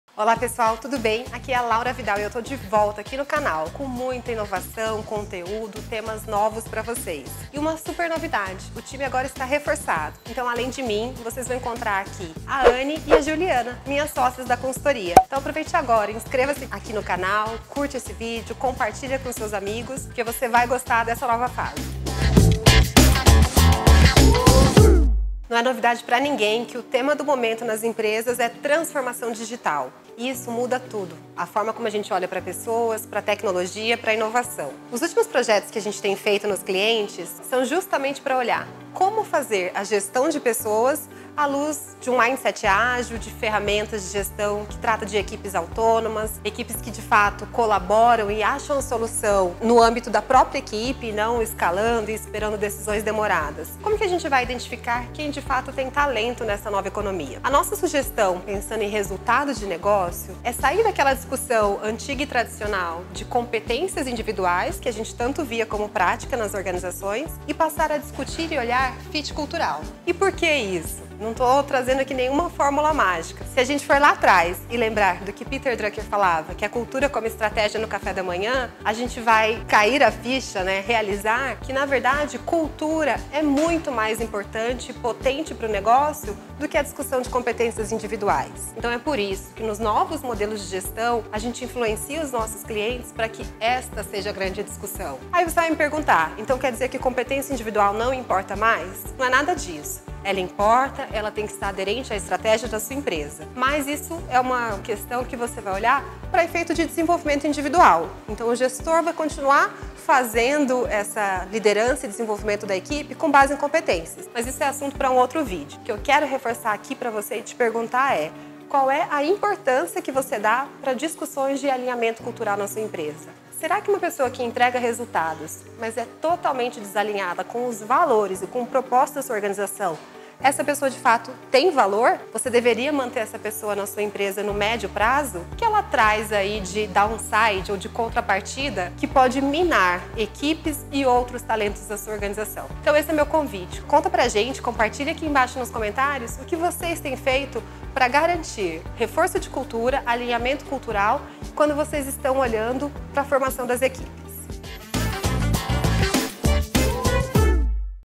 A nova gestão de talentos: suporte à transformação digital
Seus talentos estão preparados para a transformação digital? O que é mais relevante neste momento de mudança nas empresas?

Neste vídeo você vai ver nossa dica de construção de novos modelos de gestão de pessoas.